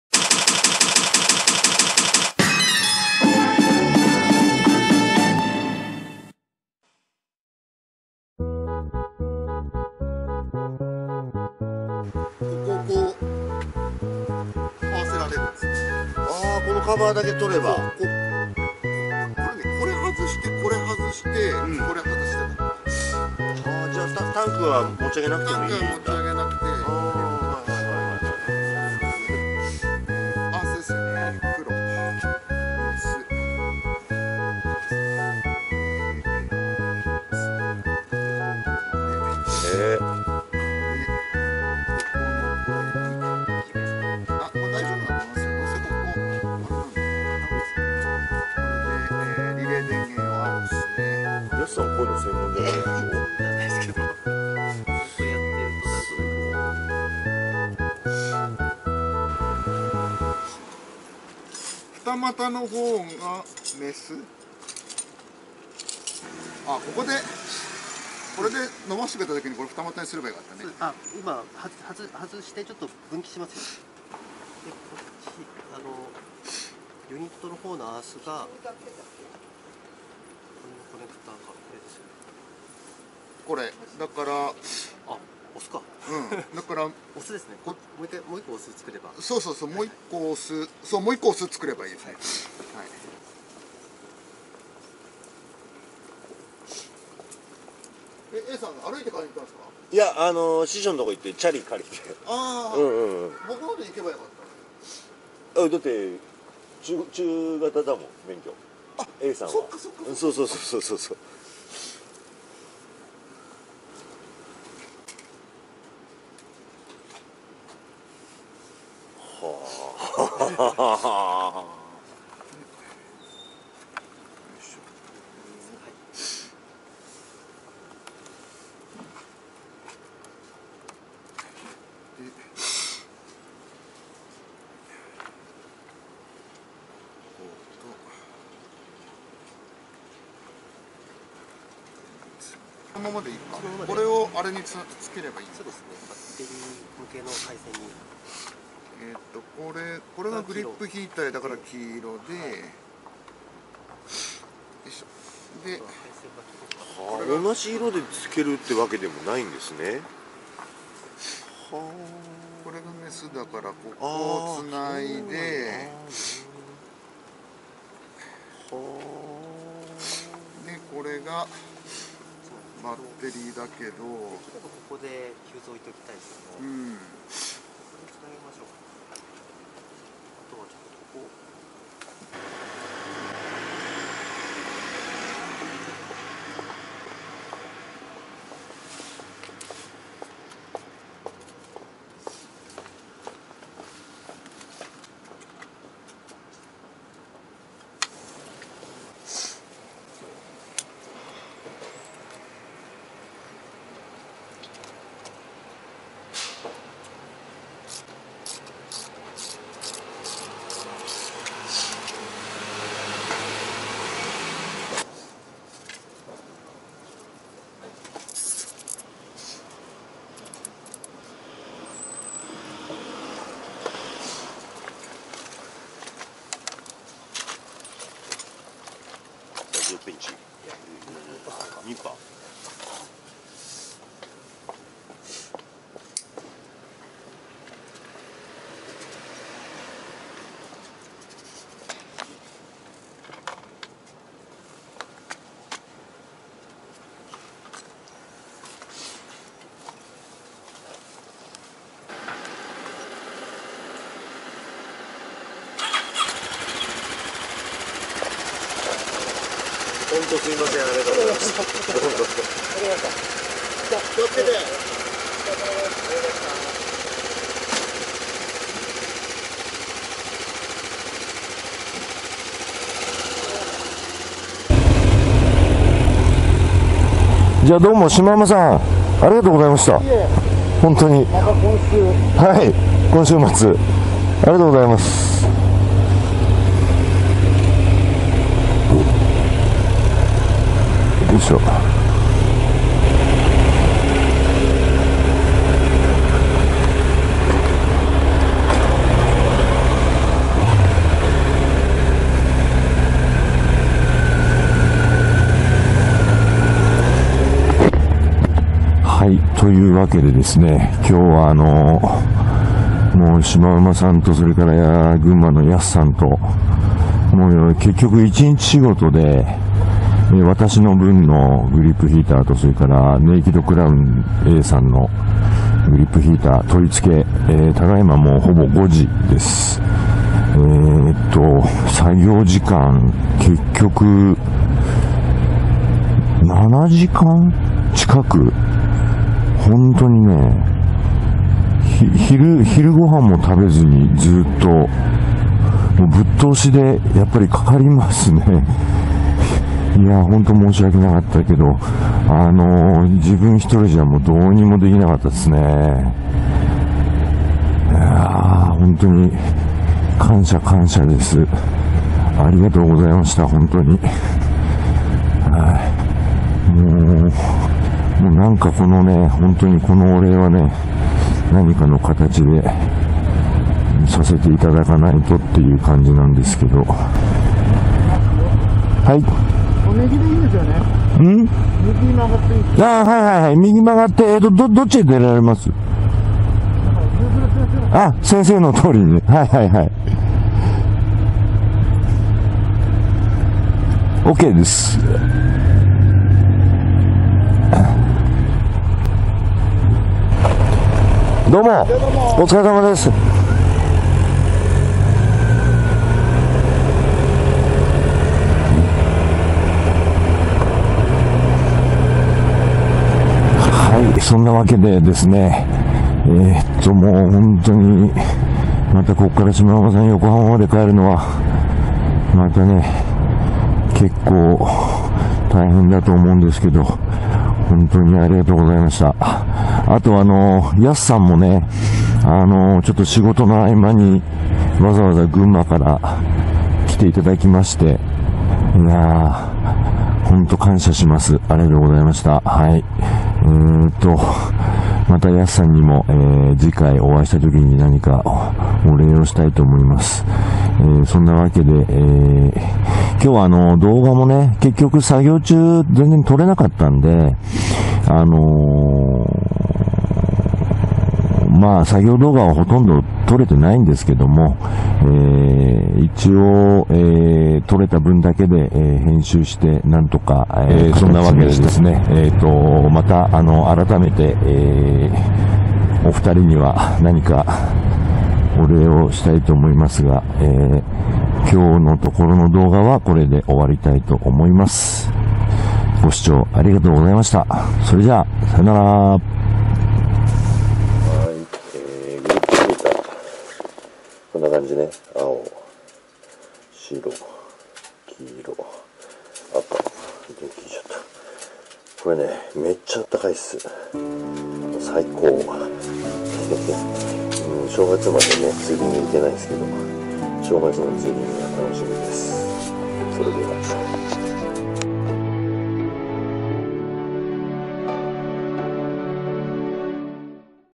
【つぶやきモトブログ17】グリップヒーター取り付け
RIDING S 縞馬さんとyasu_gunma～motogarageさんに来てもらいました
そしてNAKED CROWN　Aさんも一緒に取り付け

お世話になっている方
塔屋U-anプランニングサポート